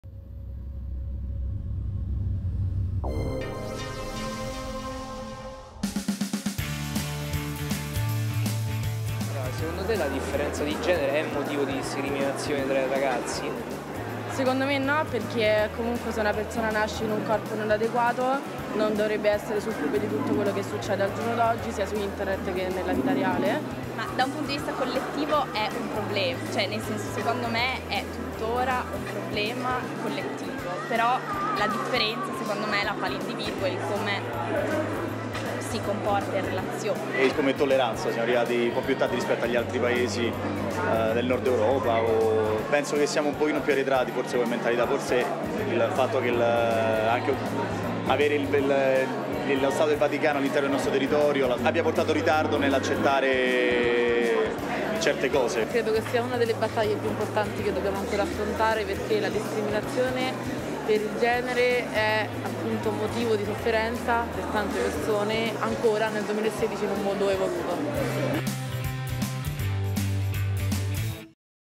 THEMES #1 - After "3 Generations" by Gaby Dellal
Intervistati dai ragazzi dello IED di Roma, gli studenti delle scuole presenti in sala riflettono sui temi e le questioni scaturite dalla visione del primo film in Concorso di Alice2016.

13|23 Ottobre 2016
Auditorium Parco della Musica - Cinema Admiral

Video realizzato dagli studenti IED ROMA Cinema & New Media - Web TV ufficiale di Alice nella Città 2016